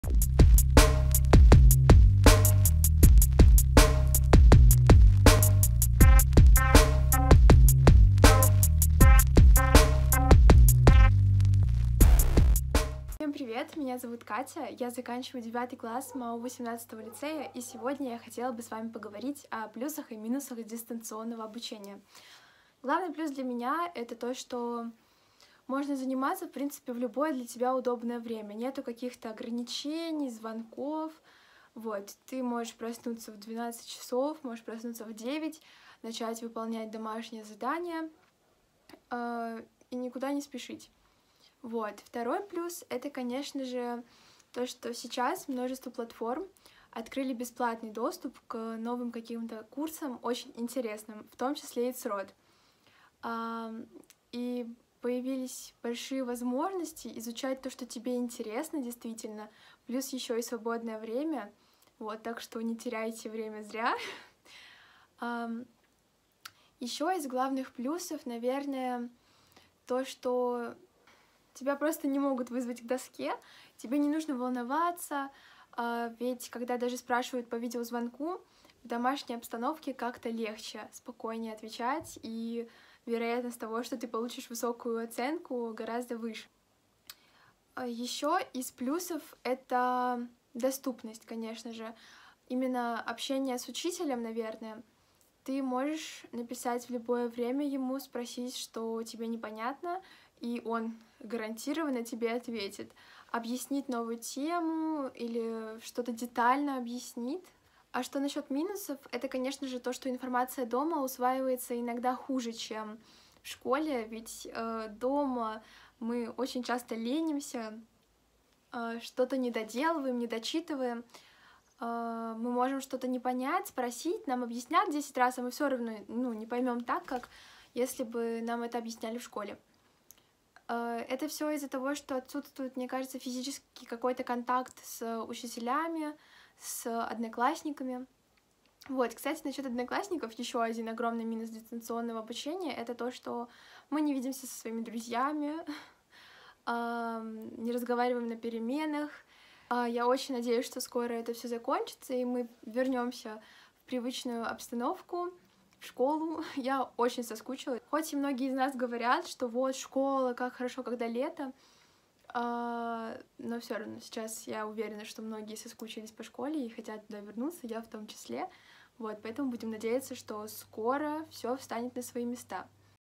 Проект «Меня слышно?». Новый ролик от наших воспитанников.
Проект «Меня слышно?» рассказывает о жизни калининградских школьников на дистанционном образовании.
О всех перипетиях нового формата обучения рассказывают, снимают и монтируют школьники сами.